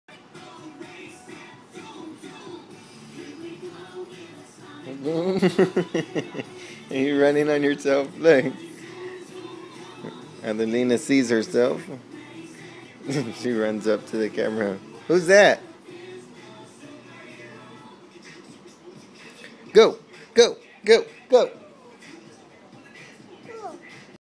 Fountain Head Hair Style
Adelina seems a little surprised at seeing herself in the camera. Is it because of her hair? :D